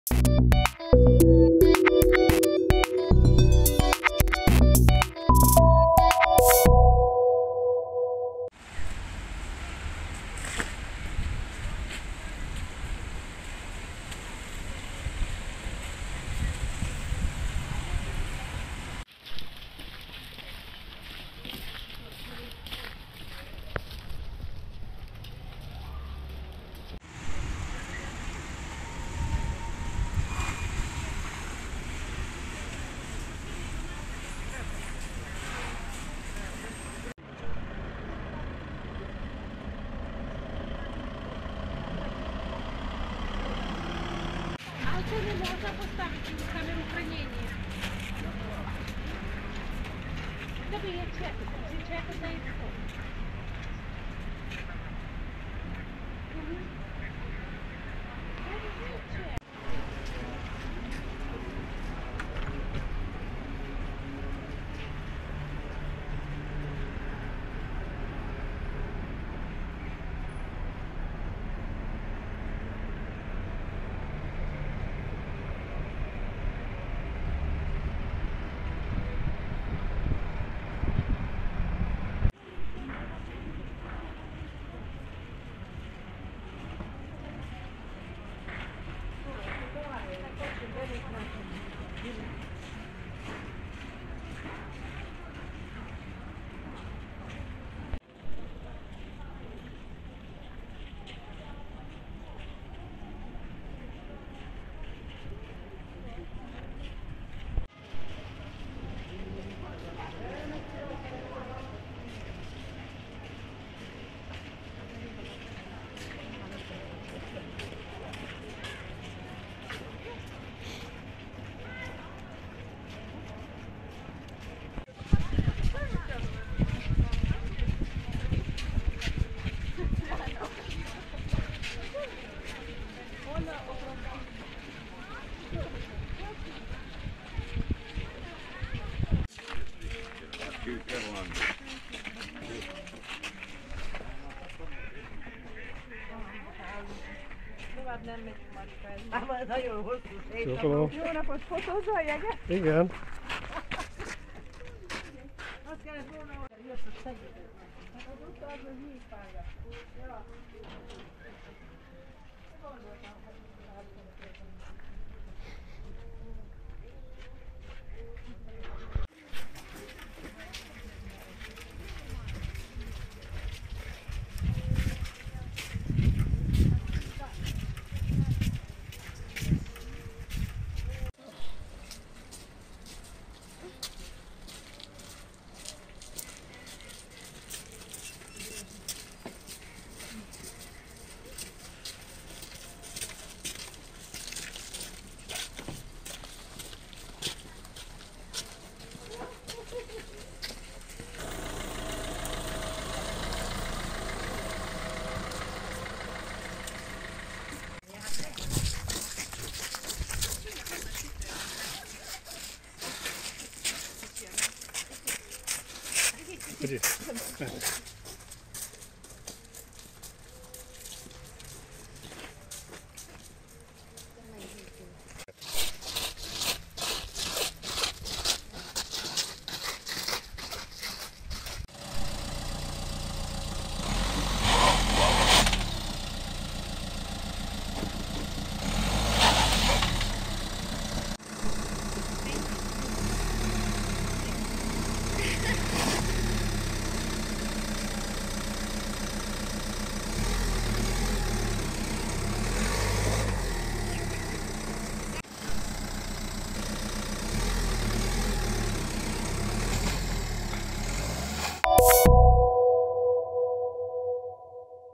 TV21 UNGVÁR - KOMMENTÁR NÉLKÜL (2020.01.09.)
TÜKÖRJÉG BORÍTJA AZ UTAKAT ÉS A JÁRDÁKAT